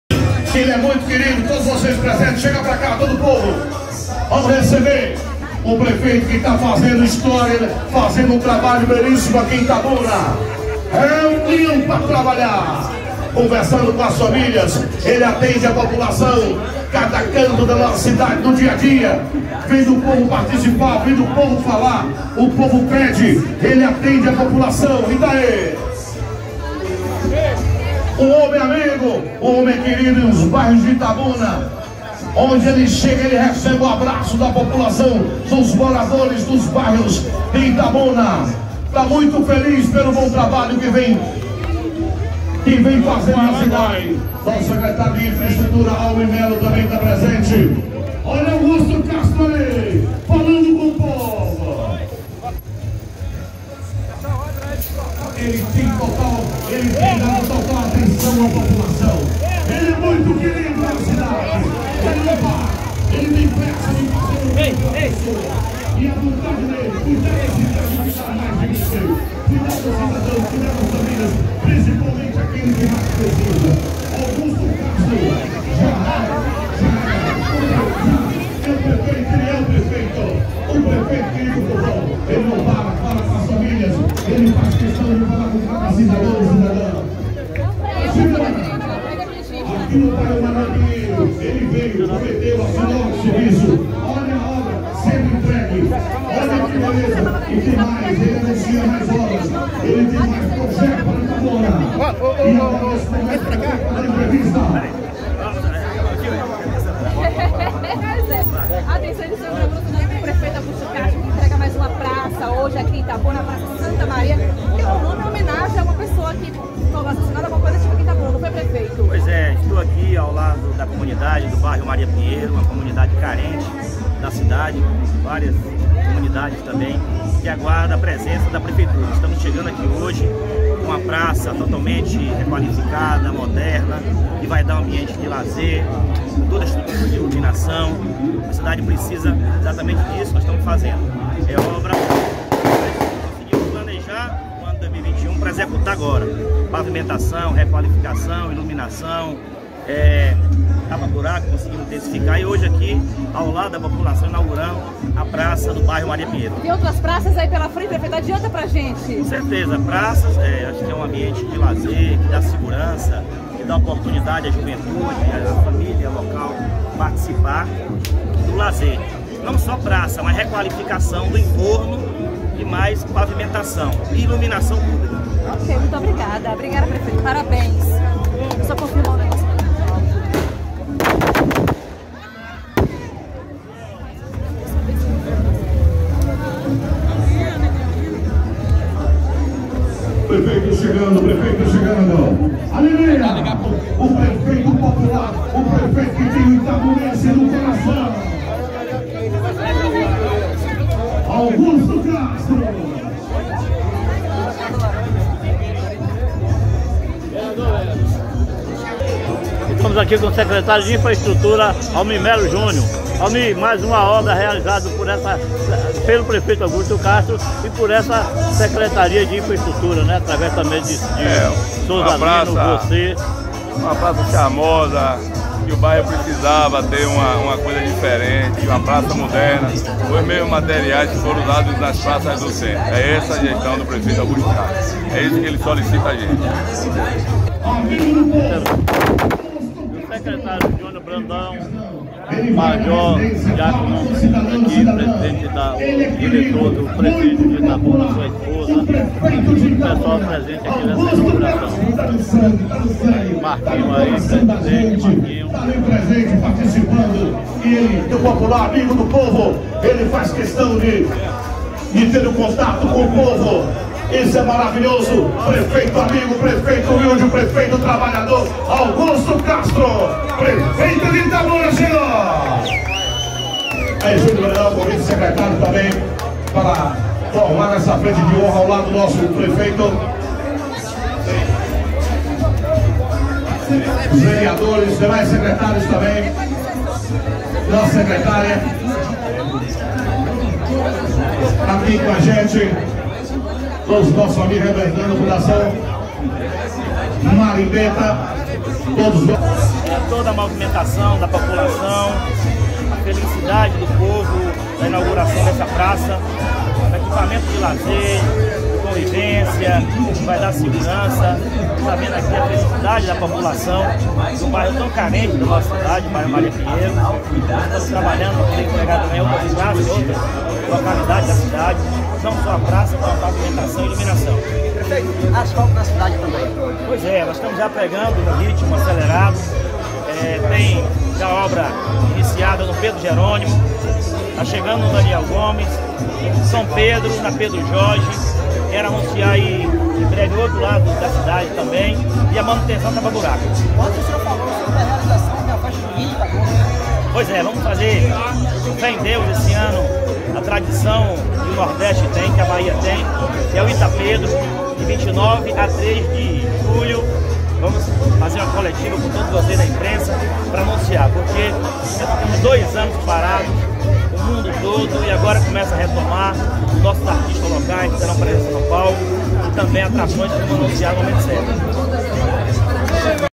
Augusto Castro Abraçado pelo povo no Maria Pinheiro
Prefeito de Itabuna entrega mais uma praça. Dessa vez no bairro Maria Pinheiro